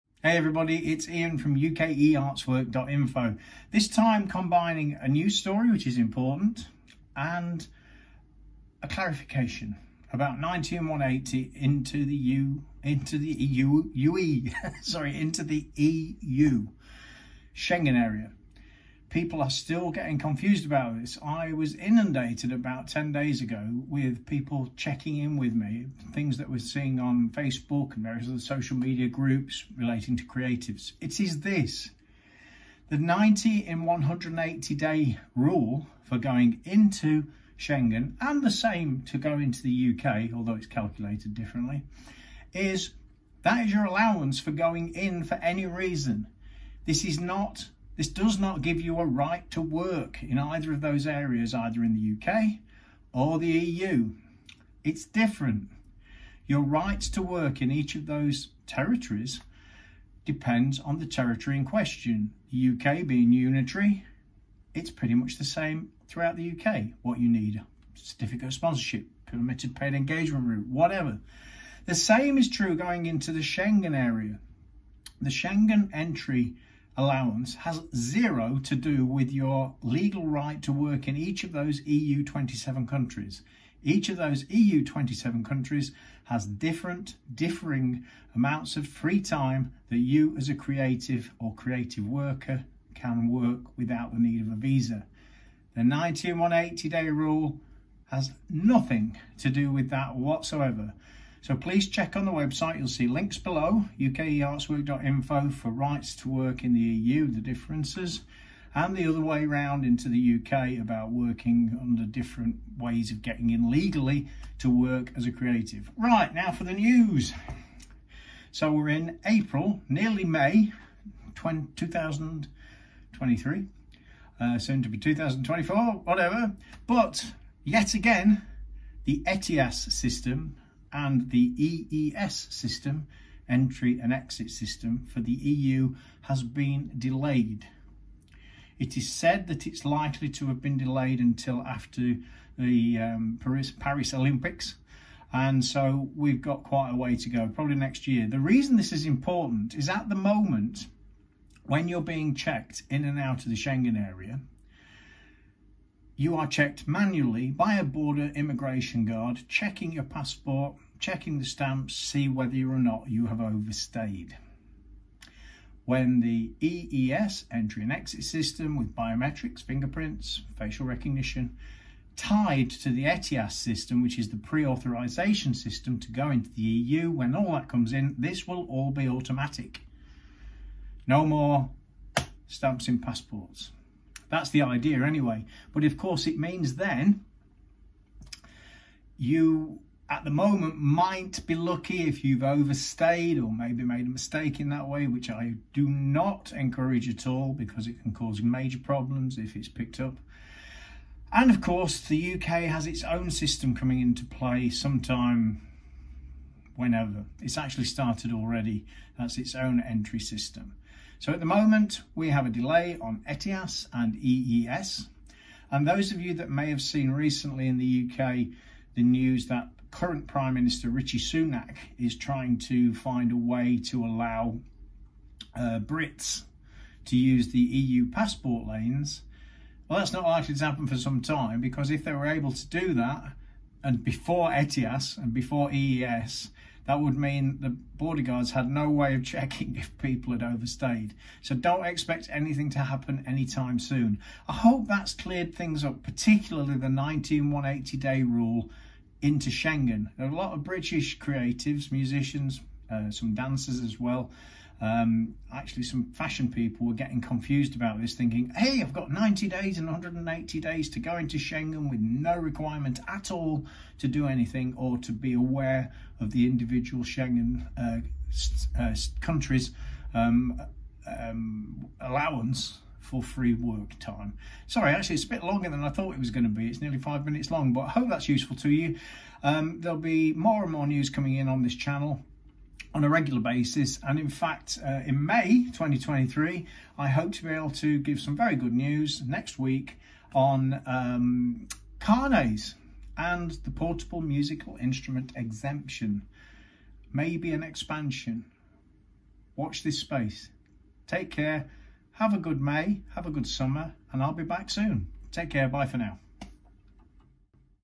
90 in 180 days NO its not a work allowance AND news on ETIAS / EES AND trailing a game changer
Schengen UK 90 days allowance nope not automatic right to work! Please check in here for a little news and a clear explanation of what the difference is plus a little news and a trailer for a bit of a game changer coming up next week for musicians in particular in Carnet and portable instruments exemption ( hint it’s good news )

Please note when I mentioned the UK system similar to ETIAS and EES not coming in fully yet I do say its started , to avoid confusion this relates to the UK rolling it out slowly at the moment to some third country nationals NOT as YET EU nationals .. I'll publish more on this when info becomes available.

SPAIN allowing UK nationals to use EU passport gates .. NB .. although this is in place at the moment , as Spain has acted unilaterally on this , my mention of not coming in anytime soon ( likely ) is that in order to do this it would have to be an EU wide agreement under the auspices of the EU not individual member states. Watch this space. :-) !

Links

Working allowances in each EU member state

Working in the UK

90 in 180 day rule counting guide